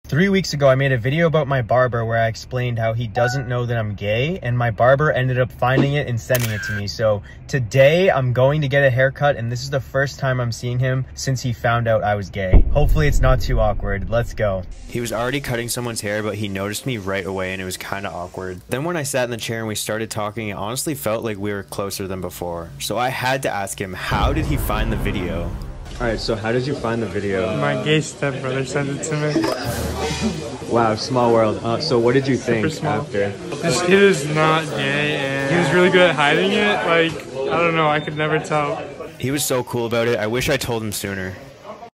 I accidentally came out to my Barber 🏳️‍🌈🫢
coming out to my barber 💈